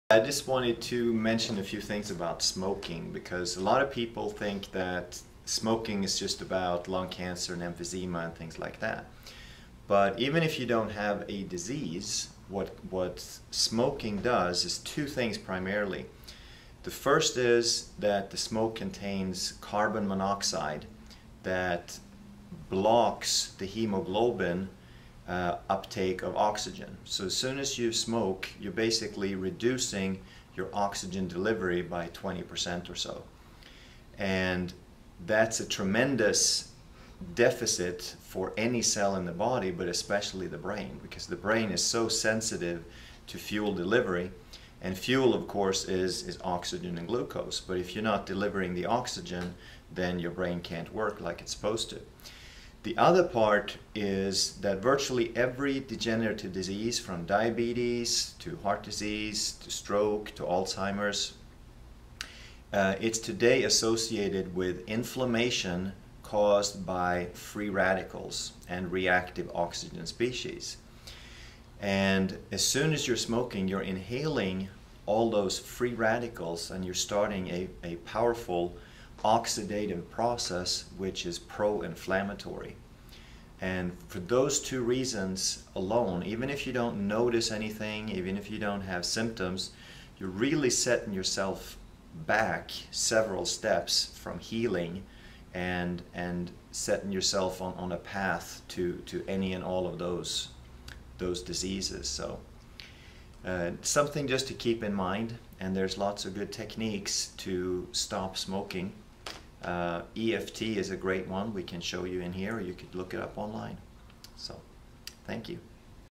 This Is What Happens To Your Brain When You Smoke
. Smoking and Your Brain. Learn the benefits of Quitting Smoking especially with smoking and your brain health. We all know now that it is not healthy to smoke. Find out what smoking does to your brain.

Wellness For Life Chiropractic
5920 Odell St